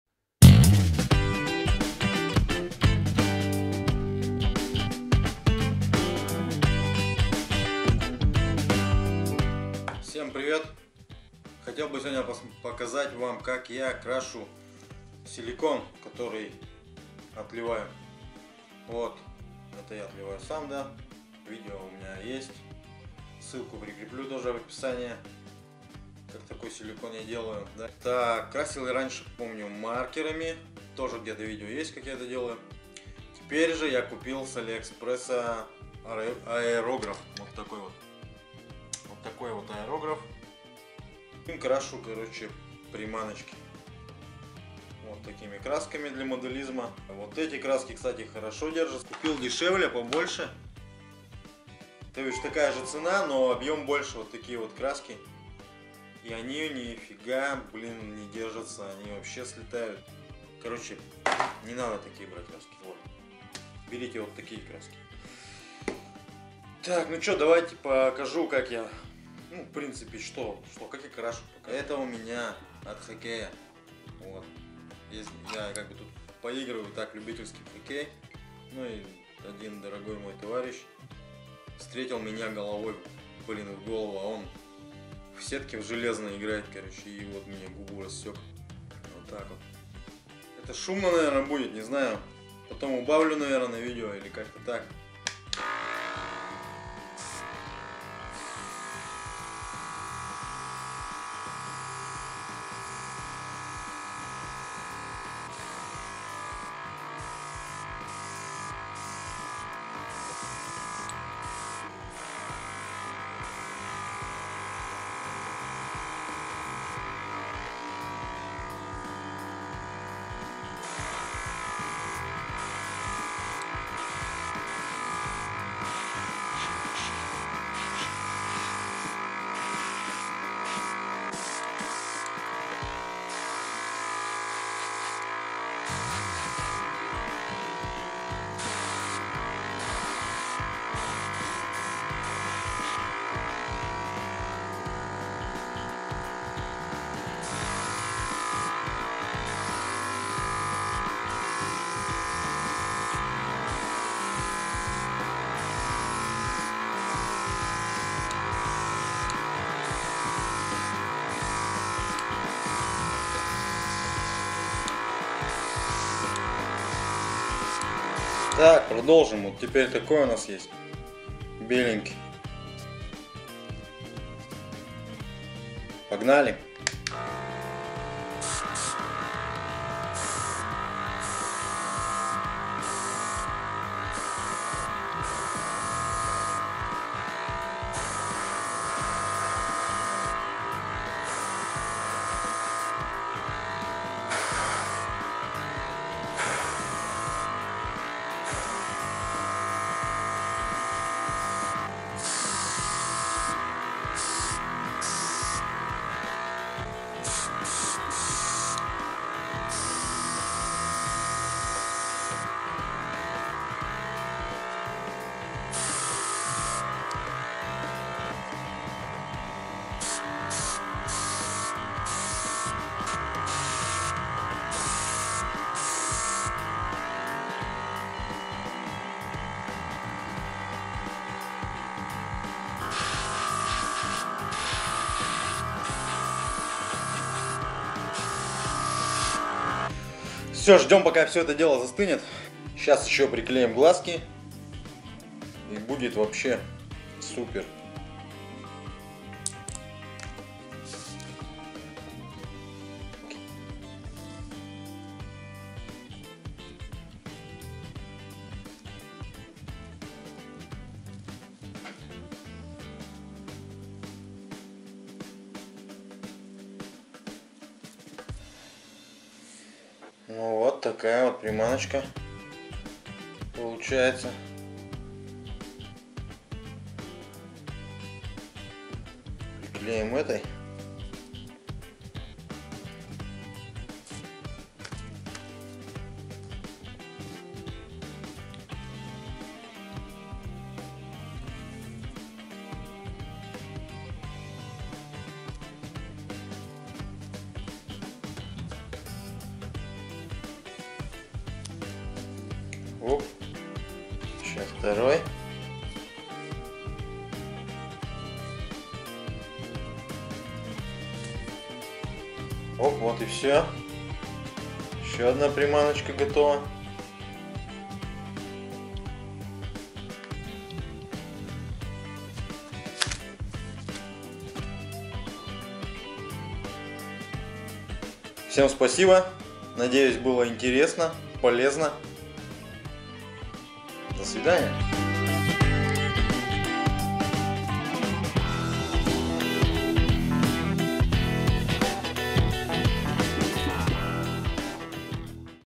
покраска силиконовых приманок аэрографом
Решил попробовать красить свои приманки аэрографом ,смотрите что из этого получилось. Ссылки ниже